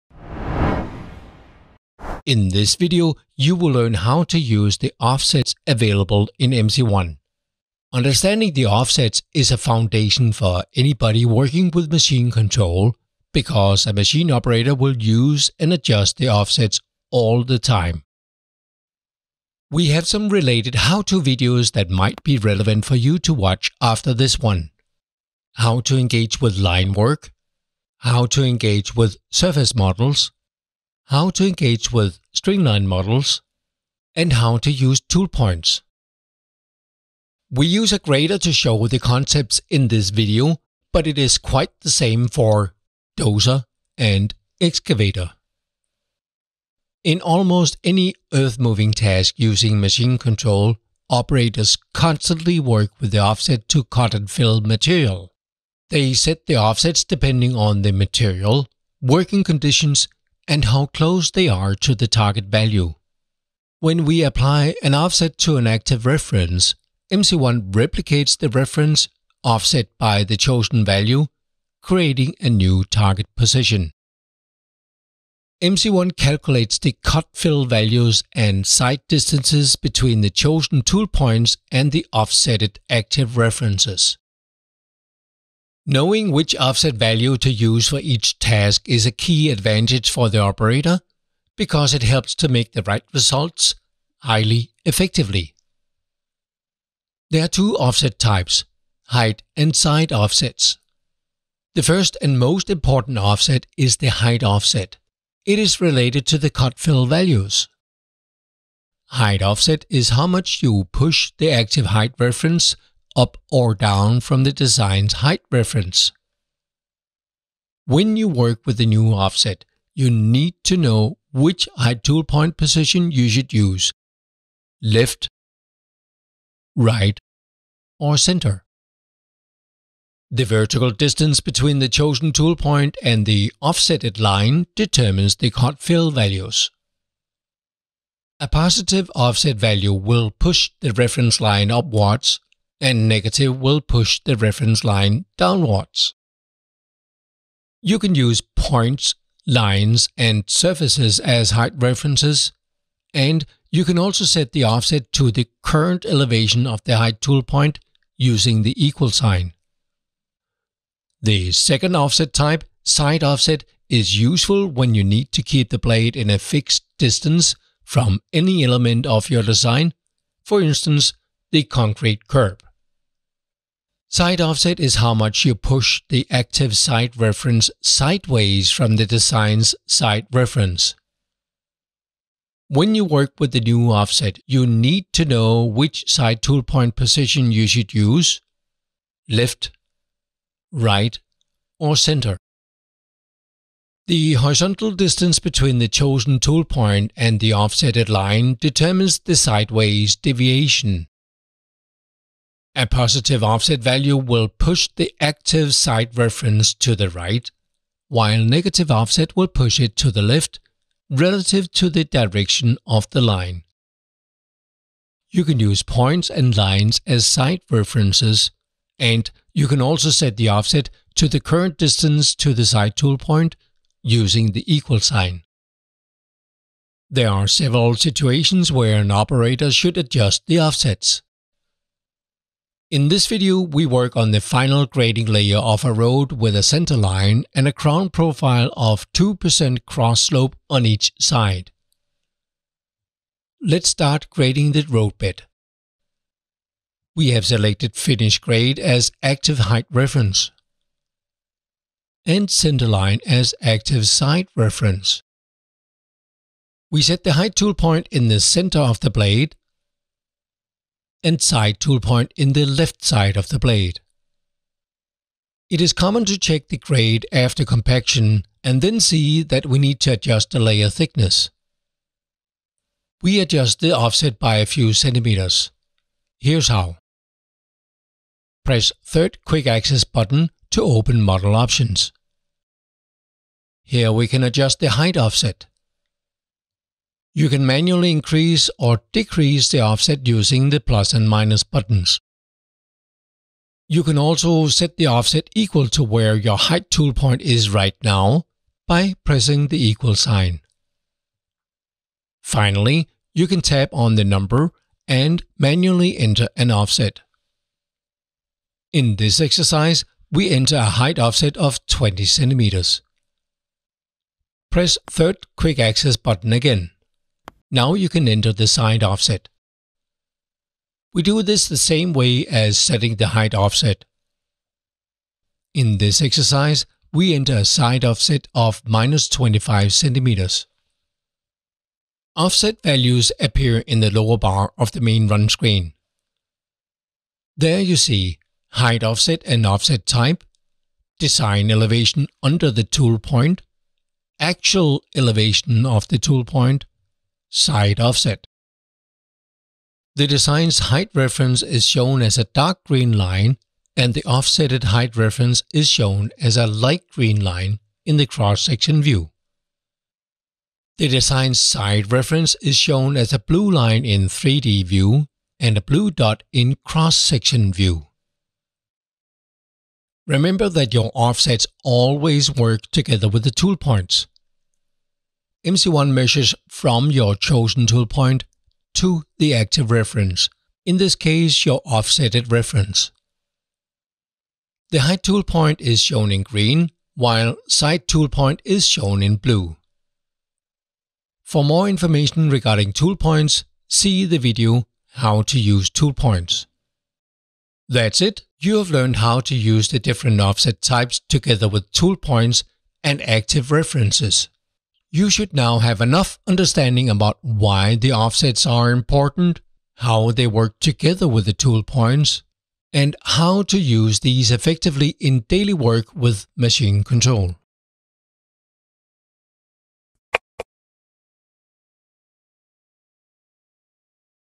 How to use offsets
You will learn how to use the offsets available in MC1. Understanding the offsets is a foundation for anybody working with machine control, because a machine operator will use and adjust the offsets all the time.
(Leica Geosystems Machine Control Academy project 031DgA-22 )